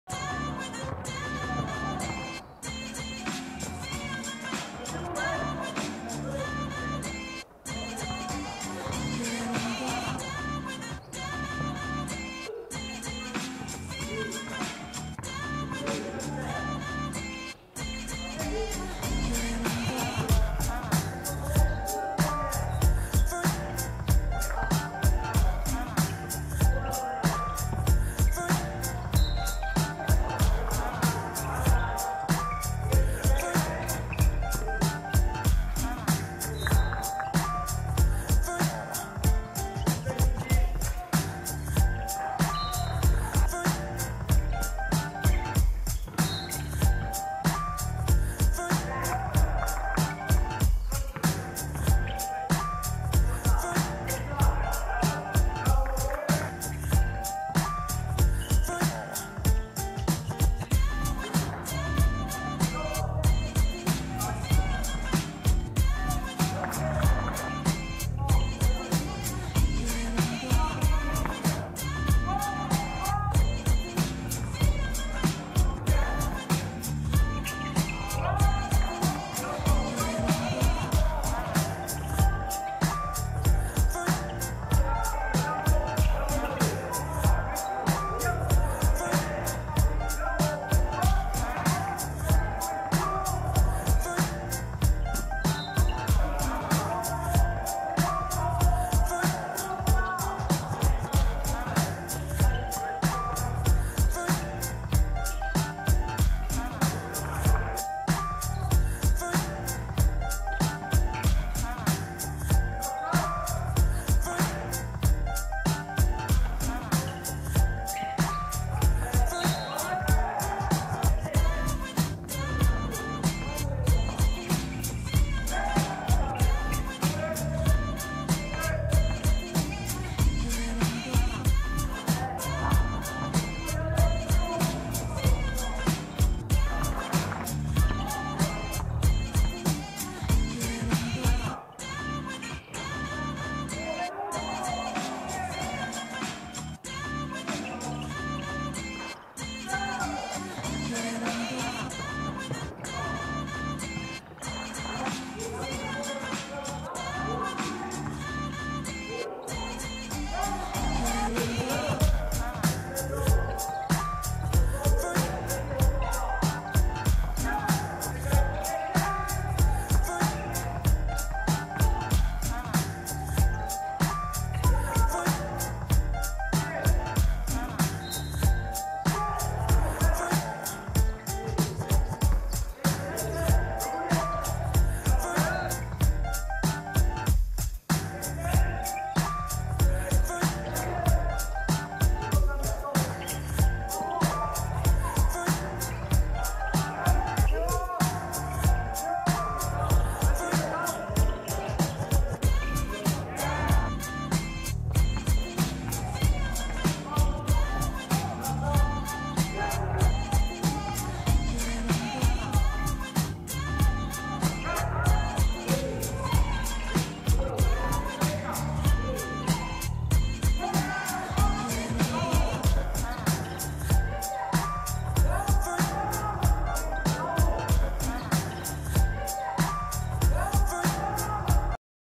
07 08 I Runda Milwaukee vs Los Angeles – Draft Camp 31 Brodnica
MILWAUKEE 59:63 LOS ANGELES
B.Młynarczyk 23pkt 9zb
W.Jaszczerski 22pkt 5zb